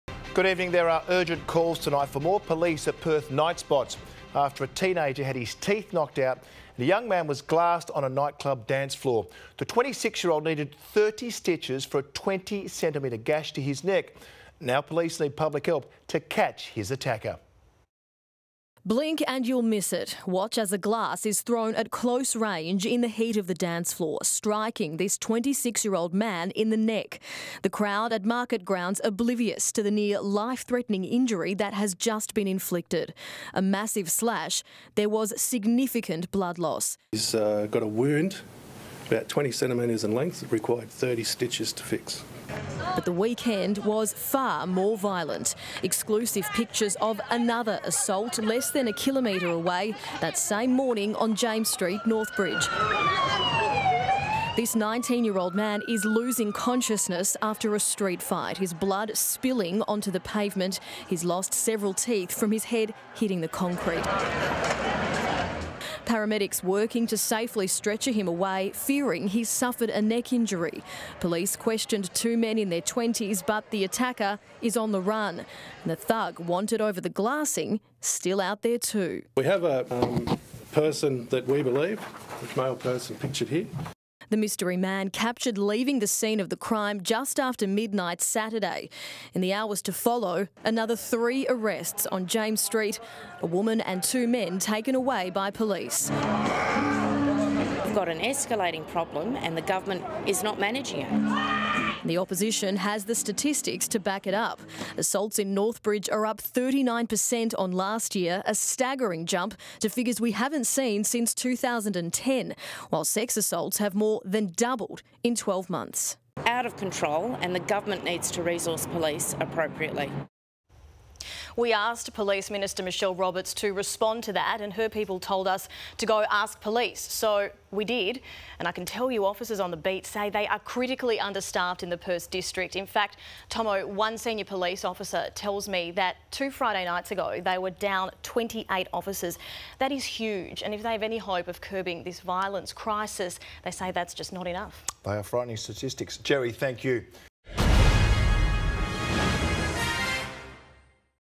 Northbridge Violence Crisis | 9 News Perth
There are calls for more police at Perth nightspots after a teenager had his teeth knocked out, and a young man was glassed on a nightclub dancefloor.

The 26-year-old needed 30 stitches for a 20cm gash to his neck.